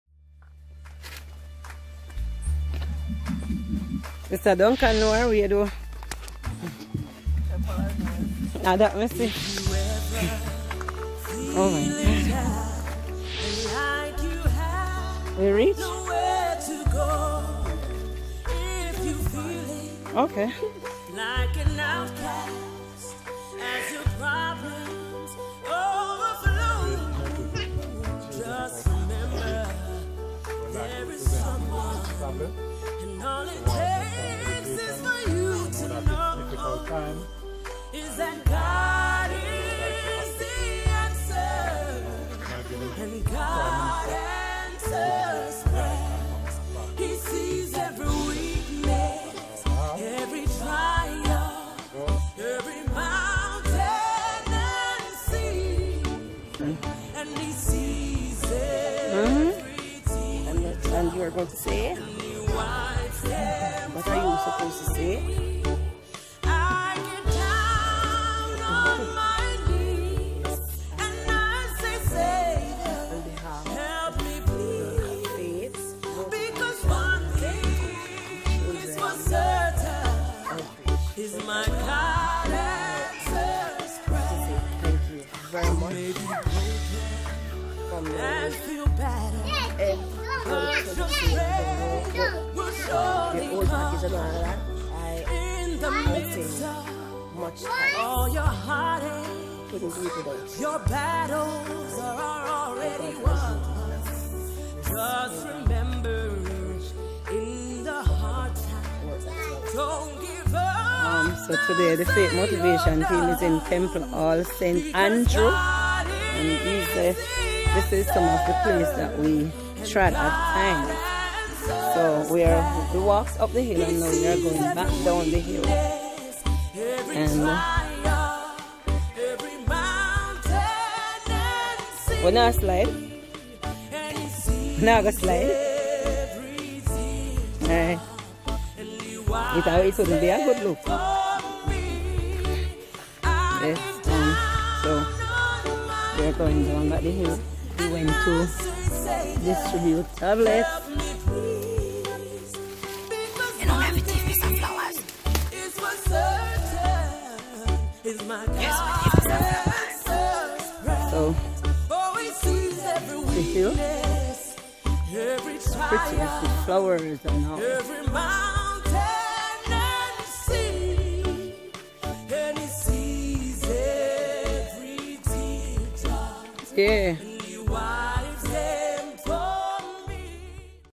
The Faith Motivation Team in Temple Hall St Andreews
Handing out tablets